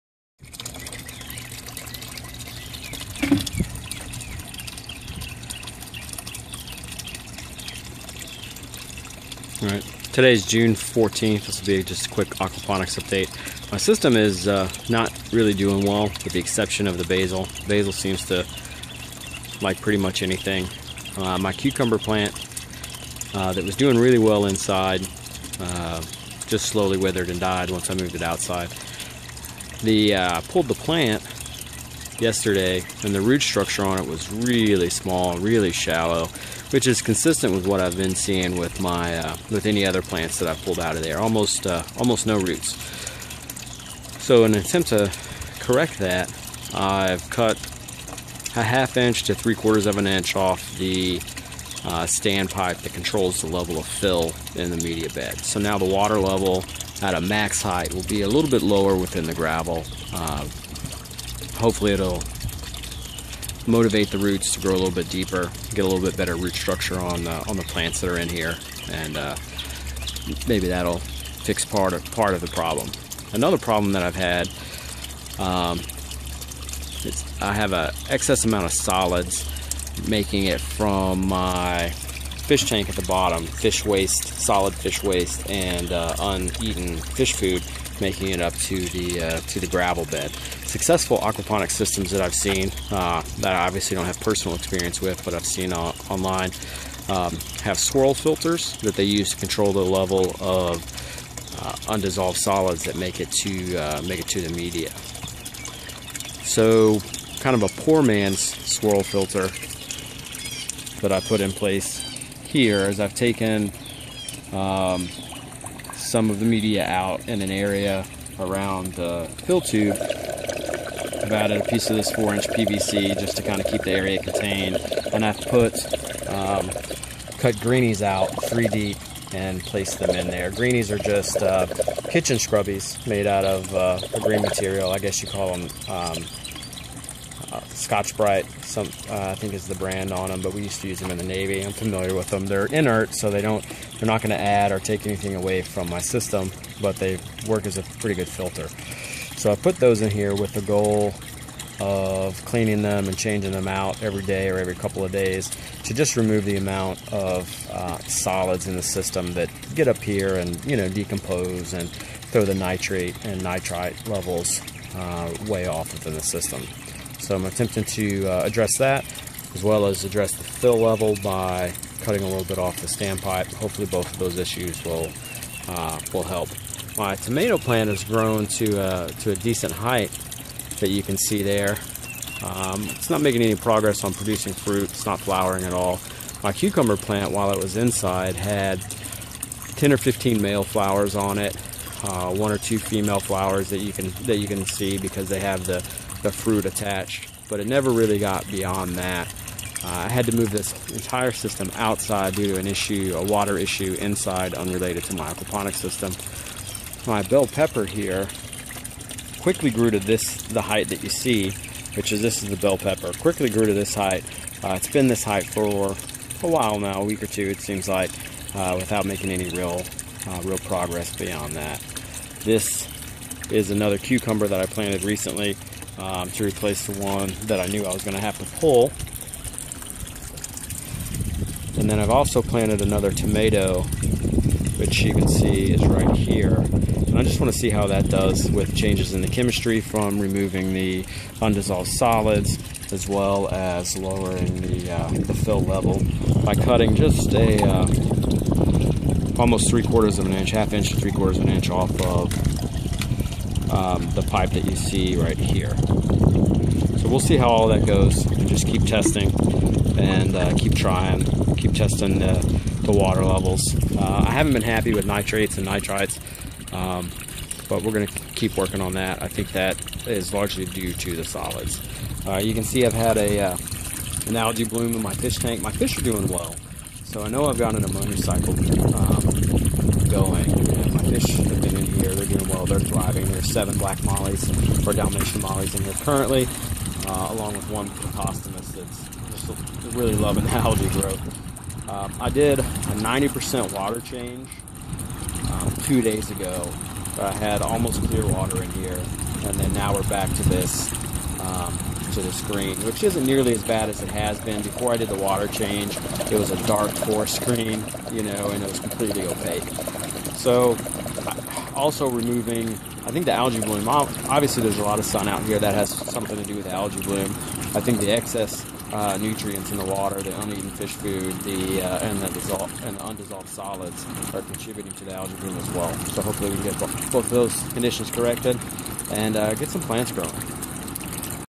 Aquaponics algea bloom etc.
Recent changes to my aquaponics system to include a change in fill level and a filter to remove solids.